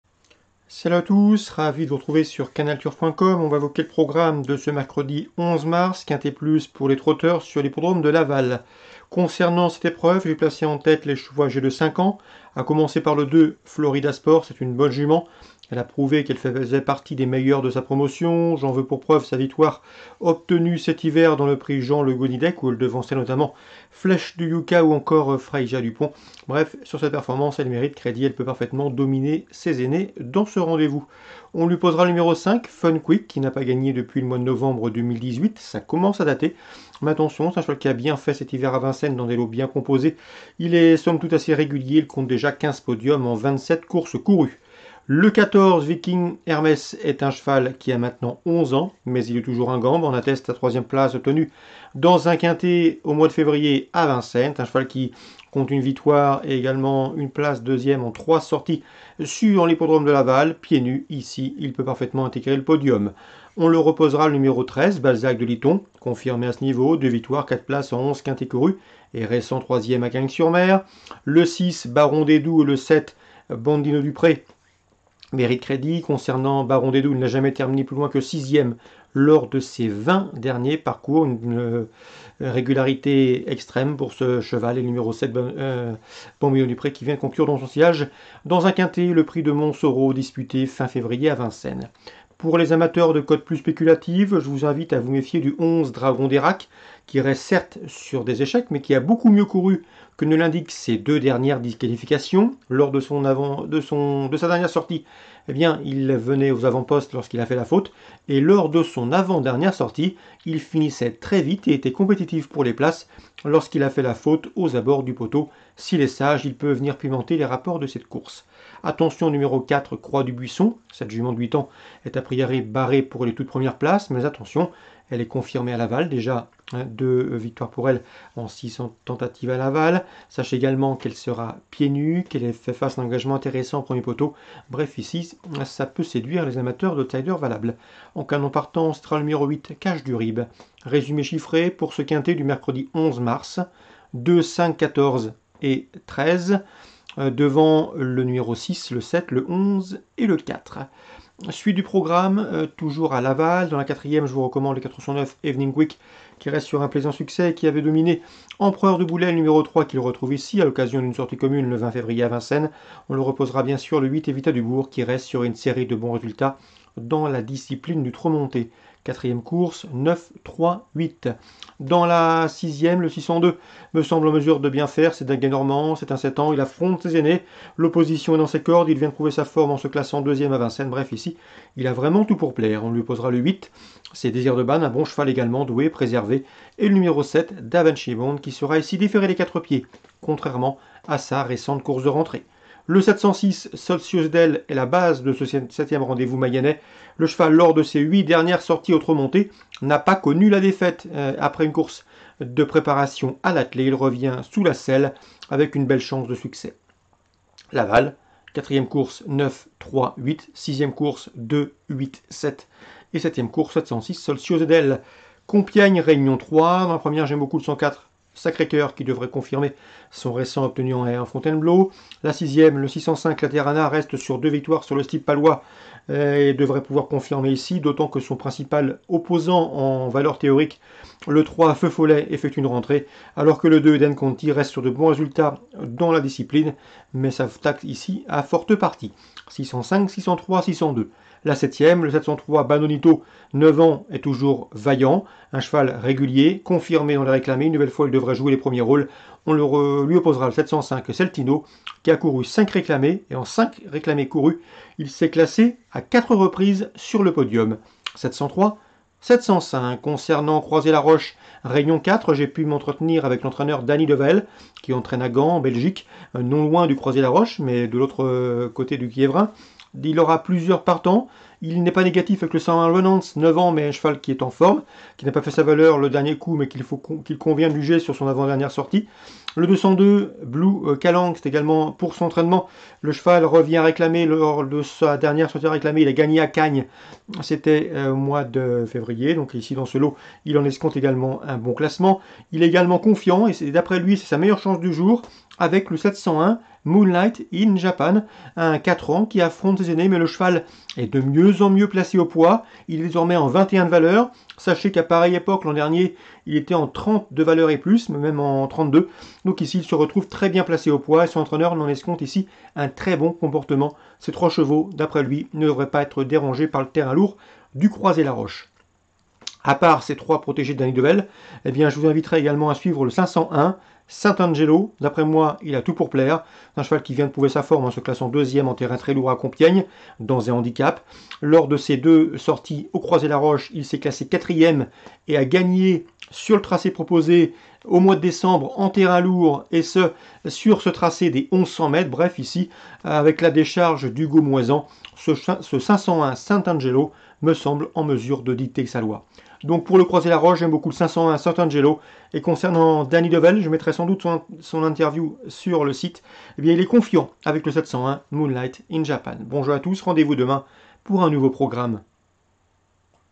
Pronostic gratuit Quinté et courses PMU du mercredi 11 mars 2020
Les pronostics vidéo du Quinté GRAND PRIX CONSEIL DEPARTEMENTAL DE LA MAYENNE et des courses PMU du Mercredi 11 mars 2020 par les journalistes de Canalturf

Quinté+ PMU GRAND PRIX CONSEIL DEPARTEMENTAL DE LA MAYENNE
13:55 - Réunion 1 Course 1 - LAVAL
TROT ATTELE - 2850m - 49000.00€